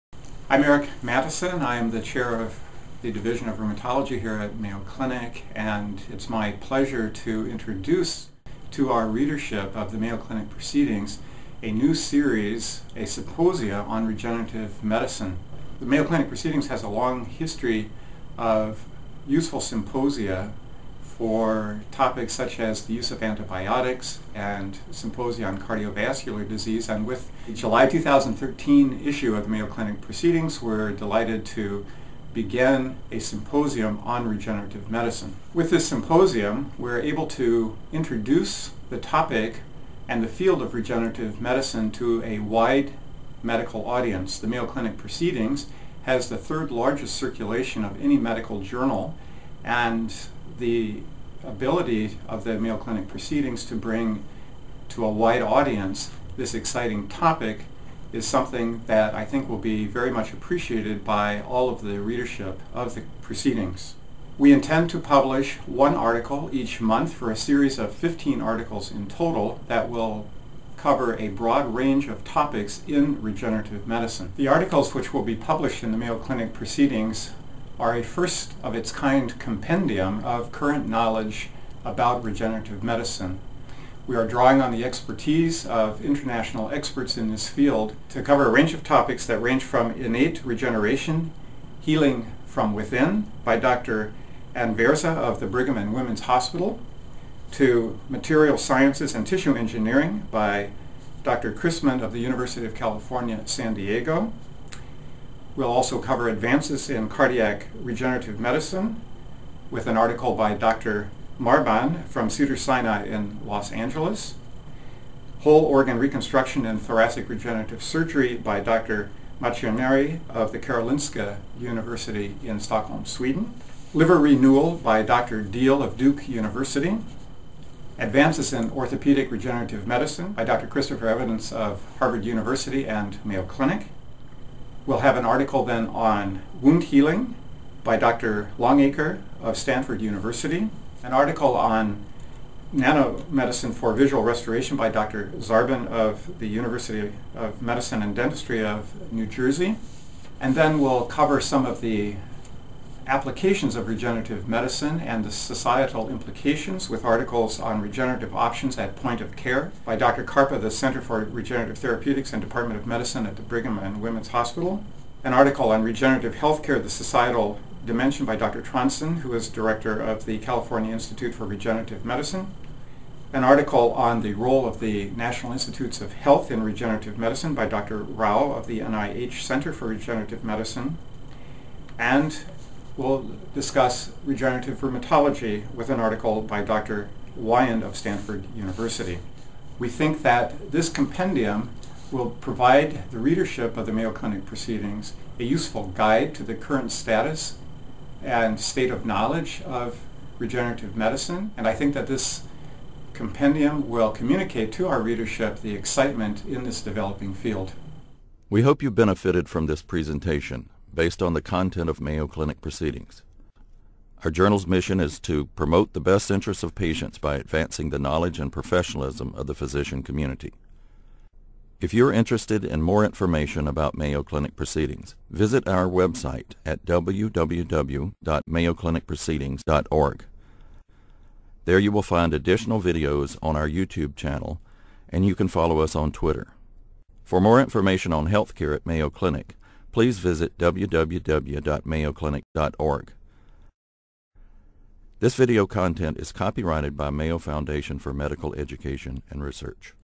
Introduction to the Symposium on Regenerative Medicine
Dr. Eric Matteson, Chair of the Division of Rheumatology at Mayo Clinic in Rochester, MN, discusses the Symposium on Regenerative Medicine launched with the July 2013 issue of Mayo Clinic Proceedings. This first-of-its-kind compendium on the developing field of regenerative medicine and surgery, presented by leading experts, highlights new vistas in treatment options for patients.